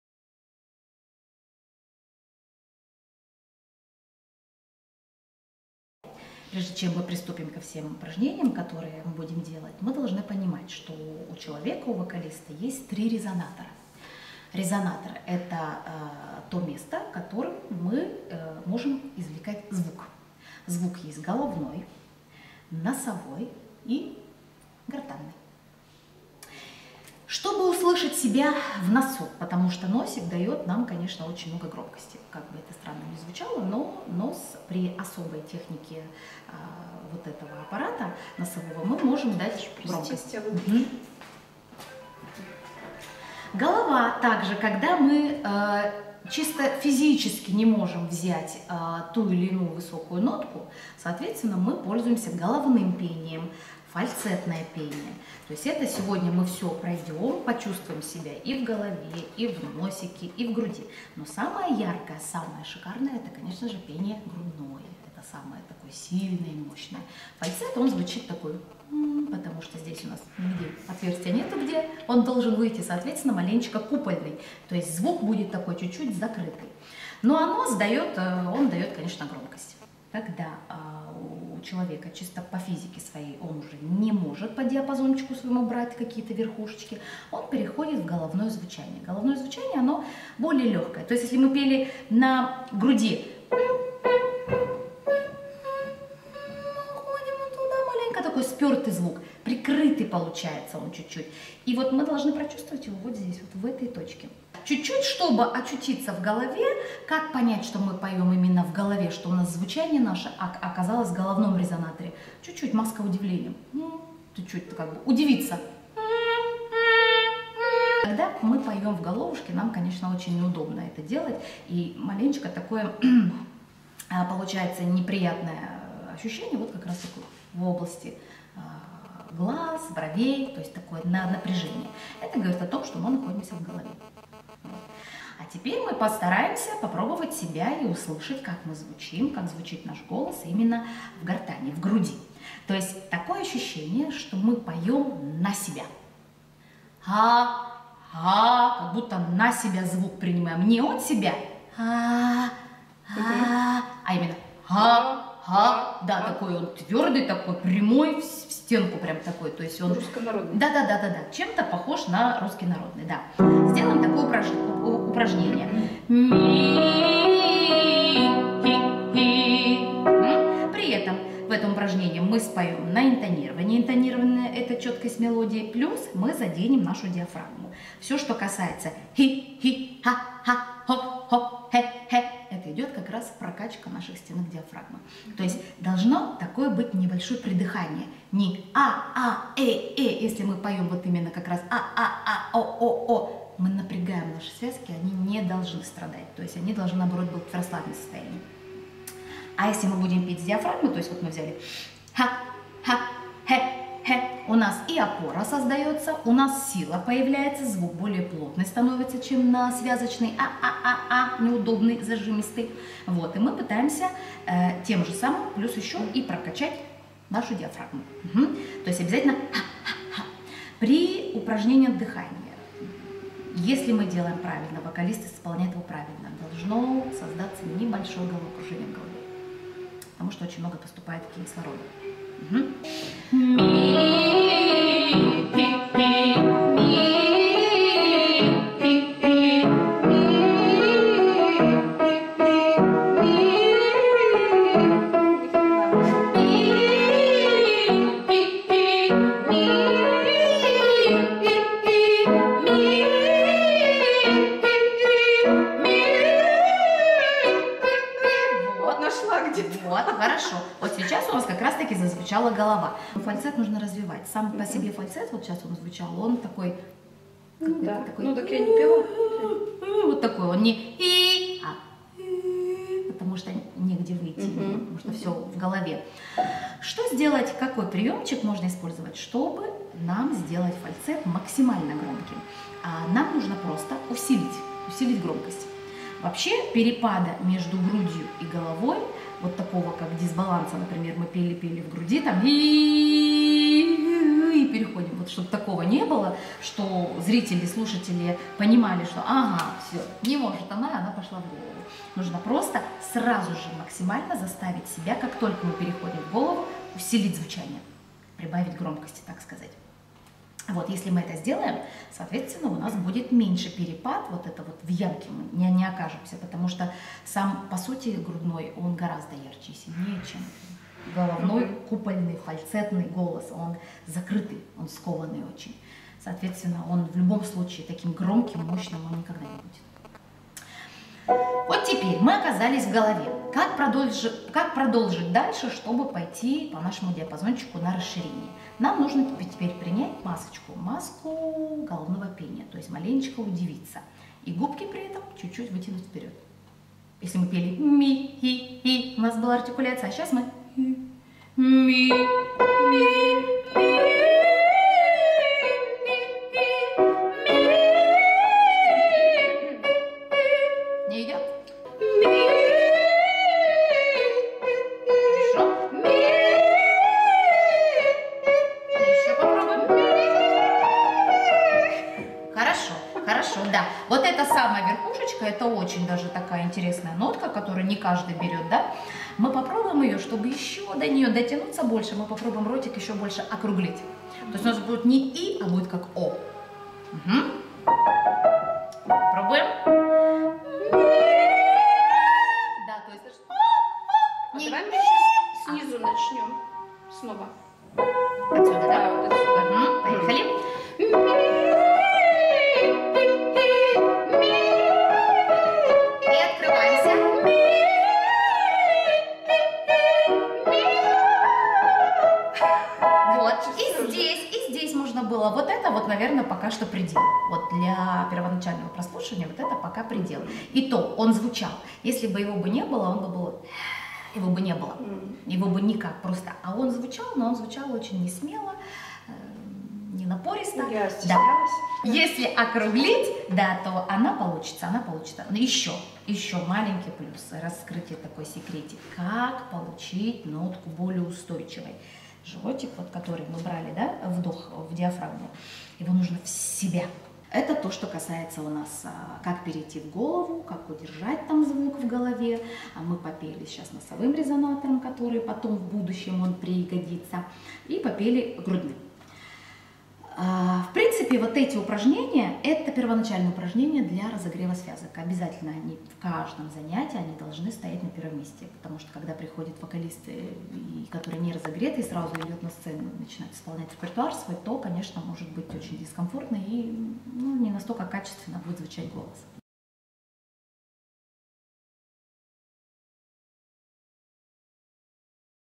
Резонаторы | Фрагмент урока вокала с педагогом по вокалу Дашей Лукиной | Интересный Котлас
Фрагмент урока с педагогом по вокалу Лукиной (Шульгиной) Дашей, руководителем Школы эстрадного Искусства в городе Котлас, Архангельской области. Даша рассказывает о головном, носовом и грудном резонаторах.